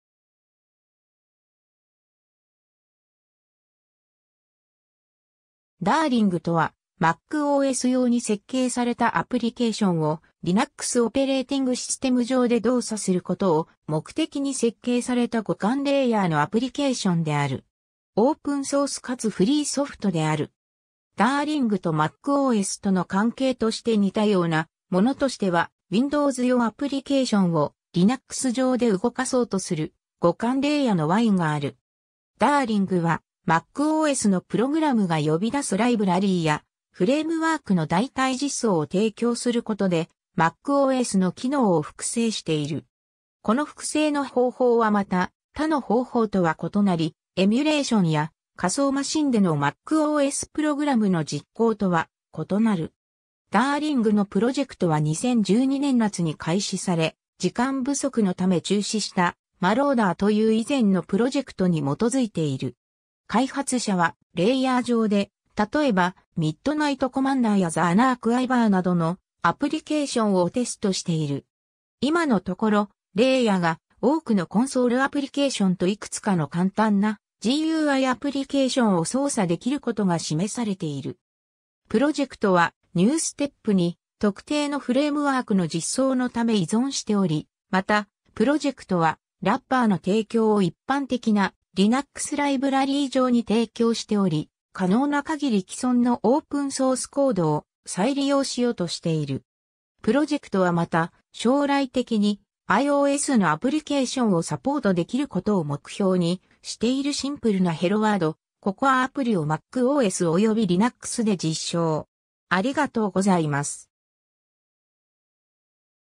Darling (ソフトウェア)
Darling(ダーリン)とは、macOS用に設計されたアプリケーションをLinuxオペレーティングシステム上で動作することを目的に設計された互換レイヤーのアプリケーションである。
オープンソースかつフリーソフトである。
DarlingとmacOSとの関係として似たようなものとしては、Windows用アプリケーションをLinux上で動かそうとする互換レイヤのWineがある。
Darlingは、macOSのプログラムが呼び出すライブラリやフレームワークの代替実装を提供することで、macOSの機能を複製している。
この複製の方法はまた、他の方法とは異なり、エミュレーションや、仮想マシンでのmacOSプログラムの実行とは異なる。
 Darlingのプロジェクトは2012年夏に開始され、時間不足のため中止したmaloaderという以前のプロジェクトに基づいている。
開発者は、レイヤー上で、たとえばMidnight CommanderやThe Unarchiverなどのアプリケーションをテストしている。
今のところ、レイヤーが多くのコンソールアプリケーションといくつかの簡単なGUIアプリケーションを操作できることが示されている。
 プロジェクトはGNUstepに特定のフレームワークの実装のため依存しており、また、プロジェクトはラッパーの提供を一般的なLinuxライブラリ上に提供しており、可能な限り既存のオープンソースコードを再利用しようとしている。
 プロジェクトはまた、将来的にiOSのアプリケーションをサポートできることを目標にしている シンプルな "HelloWord"、 CocoaアプリをmacOS（左）およびLinux（右）で実証。